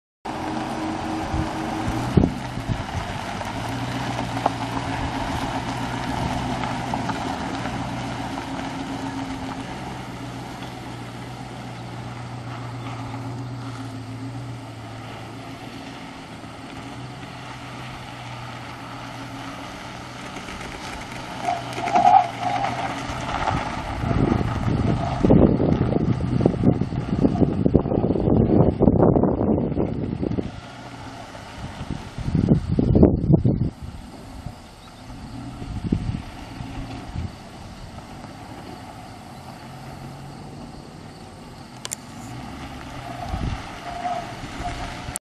2014 Dodge Charger Police for sale at auction | bidding closes October 9, 2018
2014 Dodge Charger Police
126,803 miles on odometer
5.7L V8 OHV 16V gas engine
Automatic transmission
AC and heat
Tilt
Cruise
AM/FM
225/60R18 tires
Traction control light on
Non-operational cruise
Cracked windshield
Equip# AK305
Asset# 354032
South Dakota title
2C3CDXKT5EH330547